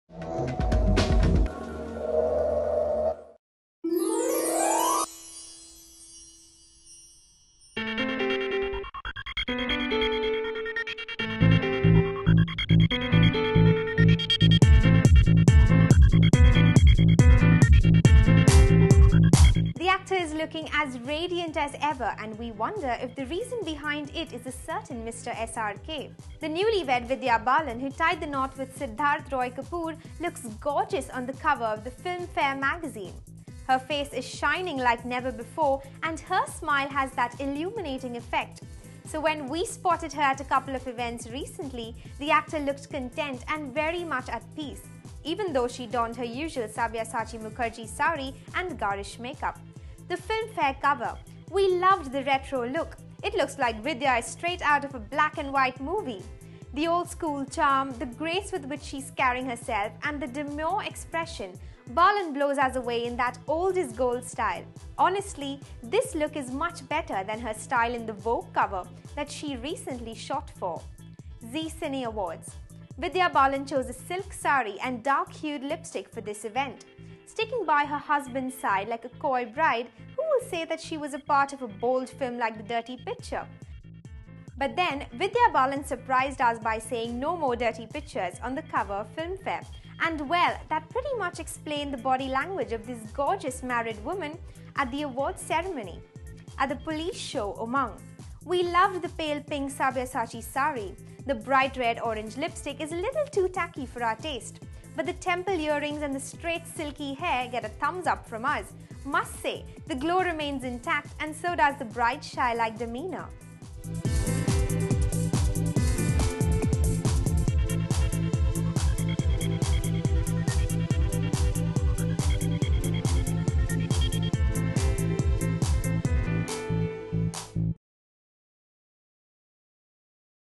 What's the reason behind Vidya Balan's glow
The actor is looking as radiant as ever and we wonder if the reason behind it is a certain Mr SRK.The newlywed Vidya Balan, who tied the knot with Siddharth Roy Kapur, looks gorgeous on the cover of the Filmfare magazine. Her face is shining like never before and her smile has that illuminating effect.